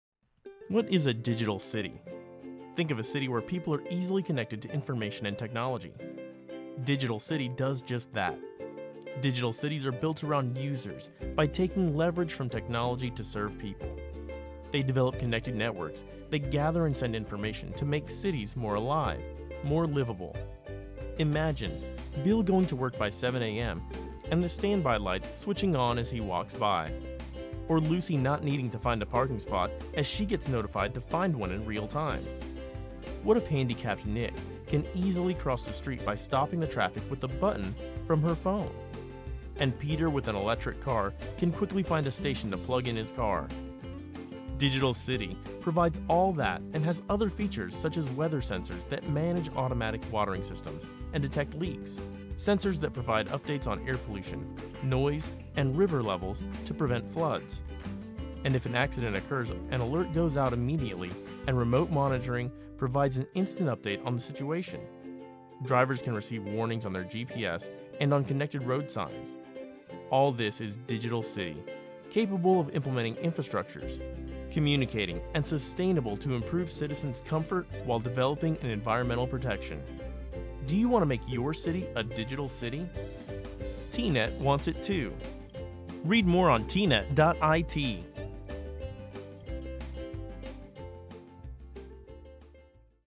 What's a Digital City?
T.net designs and implements smart projects in Digital Cities for Smart Communities and Smart Citizen. Find out our vision of an IoT cooperative World applied to your city!